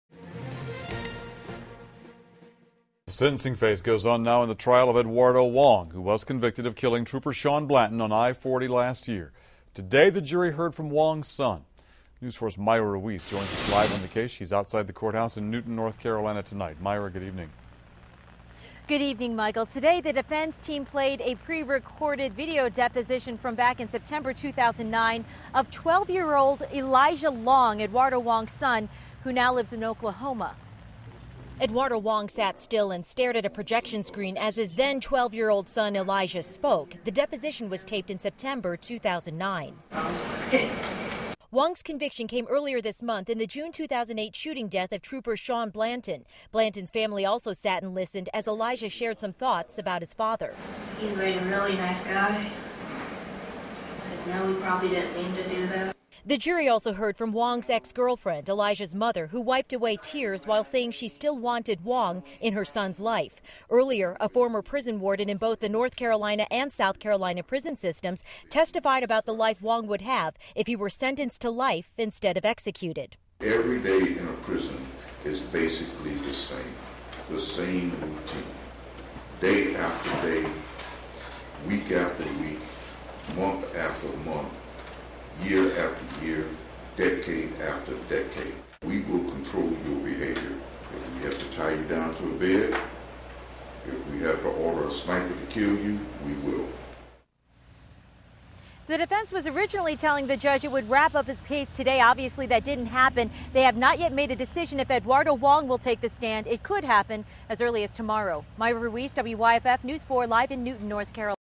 Edwardo Wong's Son Testfies
The jury heard from Edwardo Wong's son in the sentencing phase of the convicted trooper killer's trial.